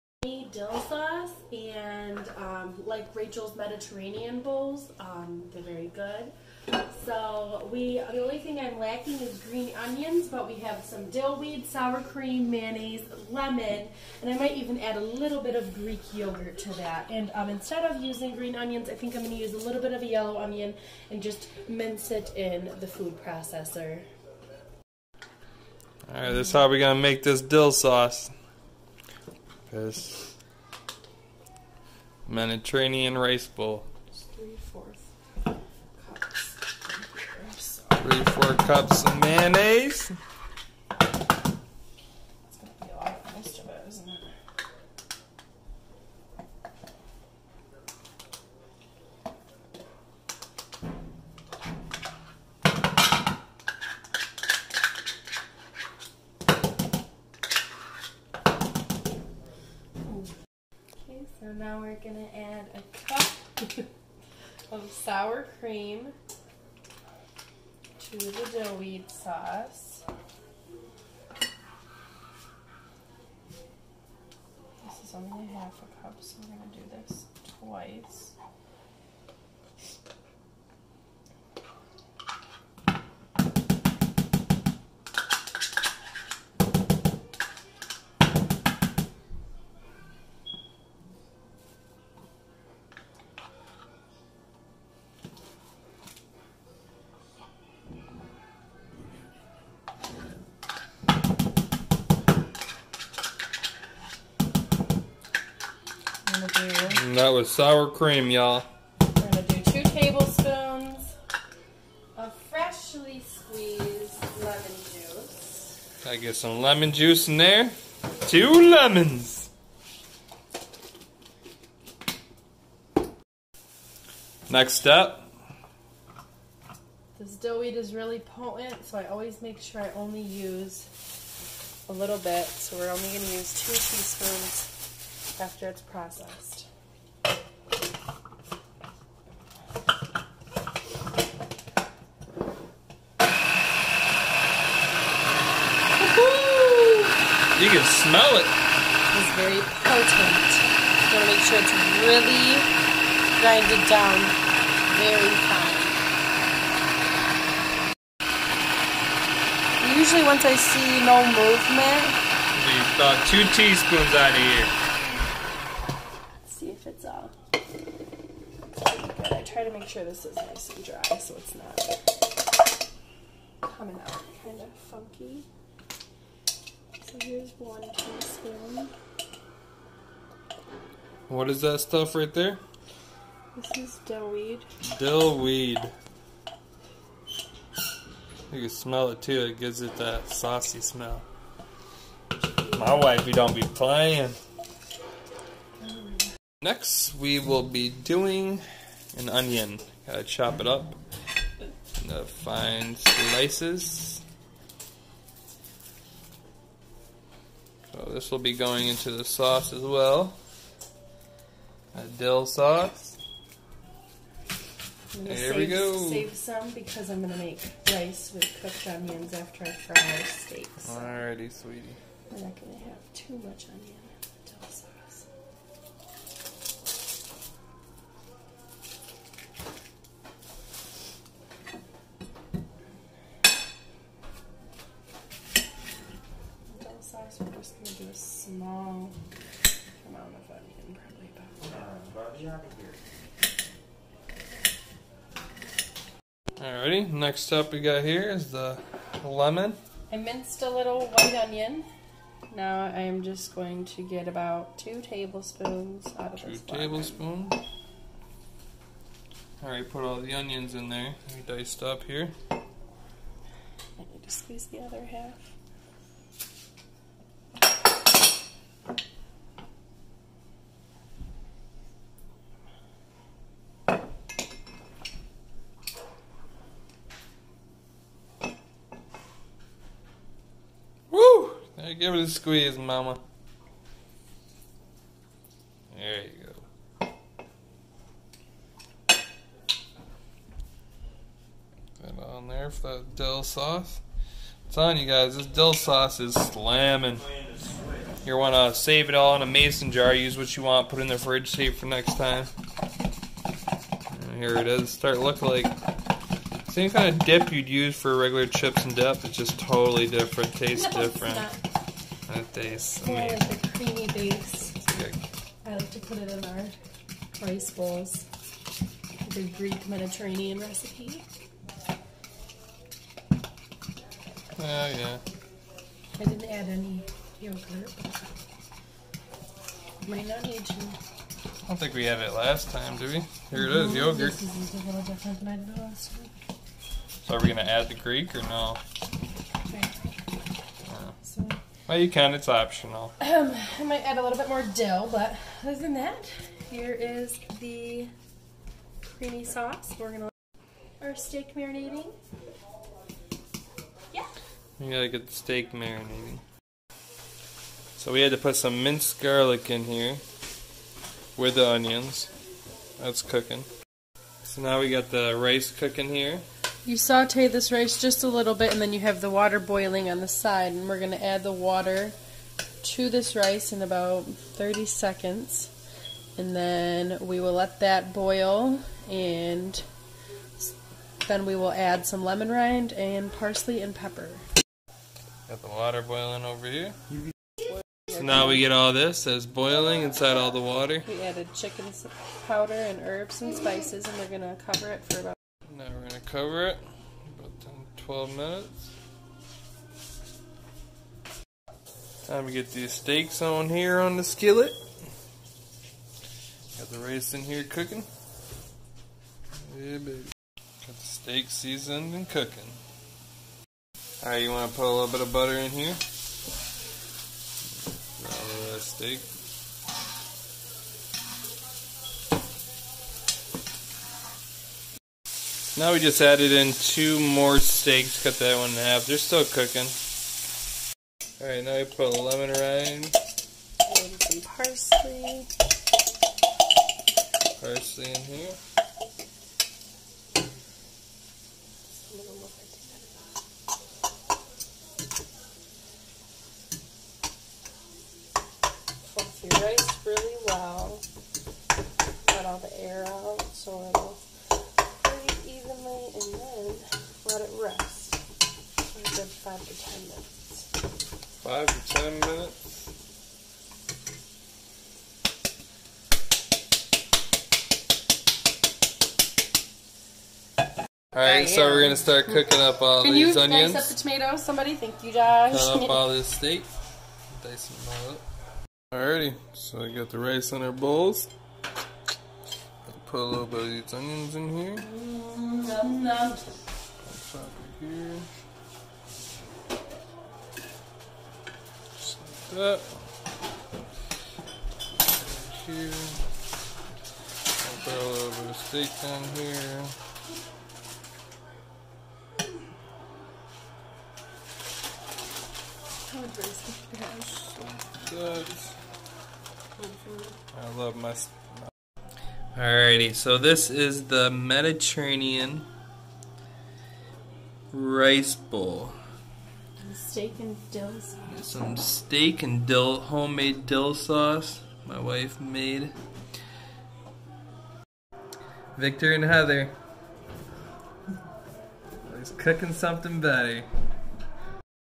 Mediterranean Rice Bowls
(Victor and Heather ) cooking things together!!
Husband n wife , Jamestown NY ❤️❤️❤️👌